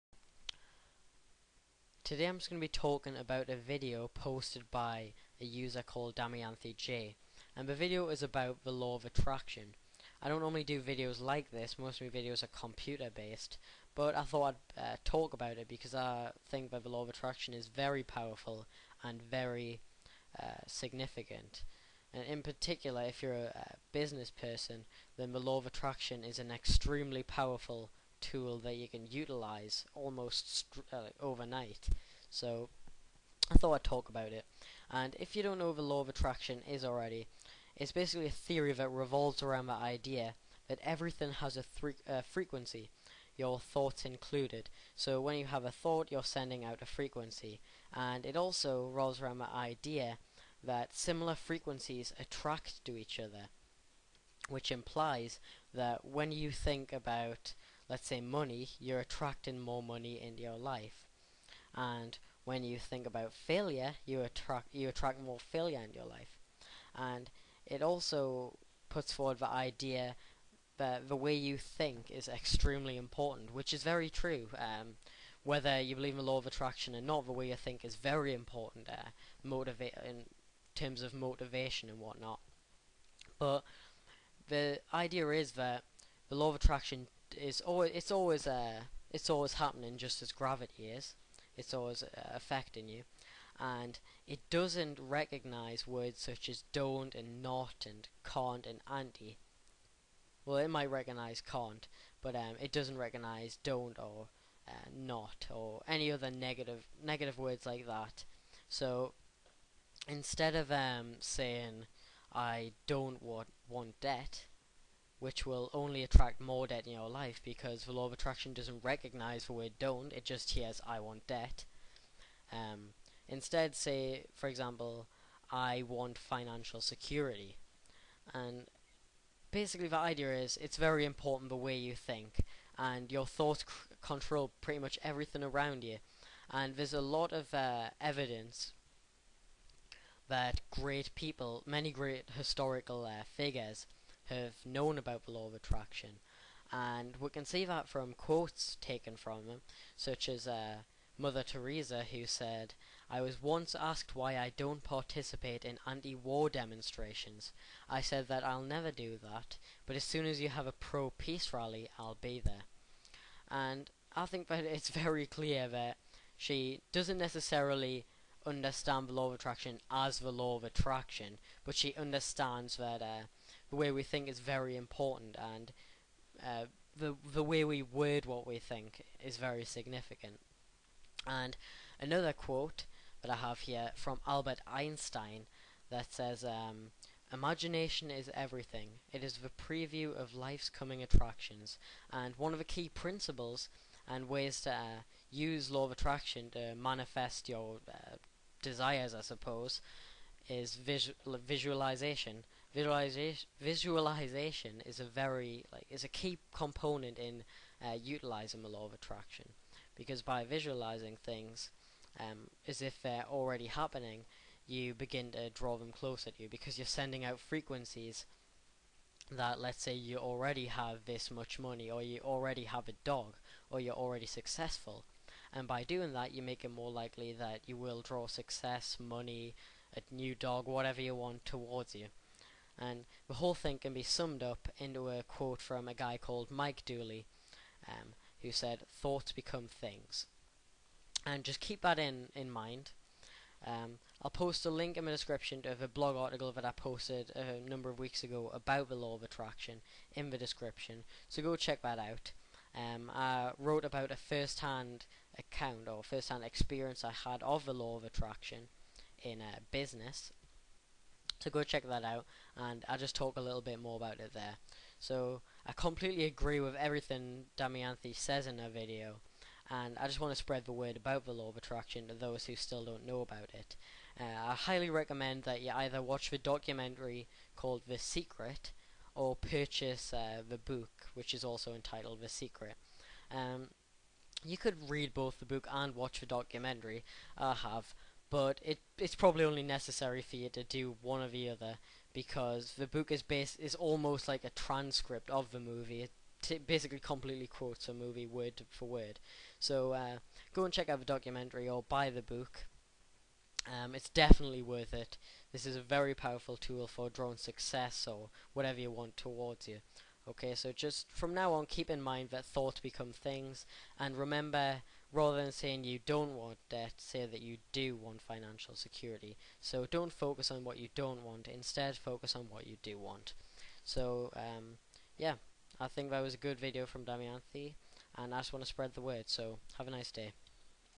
The Law of Attraction aka. The Secret
I decided to make this video after watching Damayanthi's post about the Law of Attraction, I hope you find it useful.